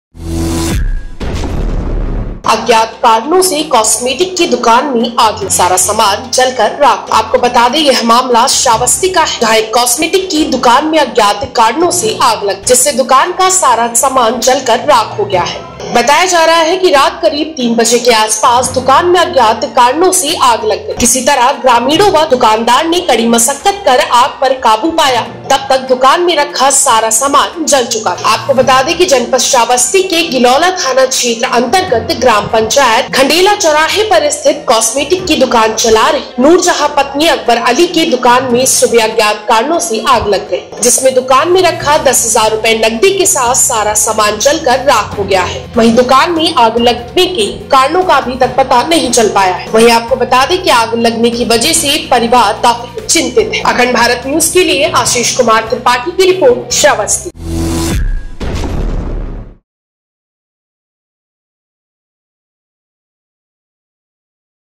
अज्ञात कारणों से कास्मेटिक की दुकान में लगी आग.....
आशीष कुमार त्रिपाठी -श्रावस्ती
Install The Akhand Bharat News Android App: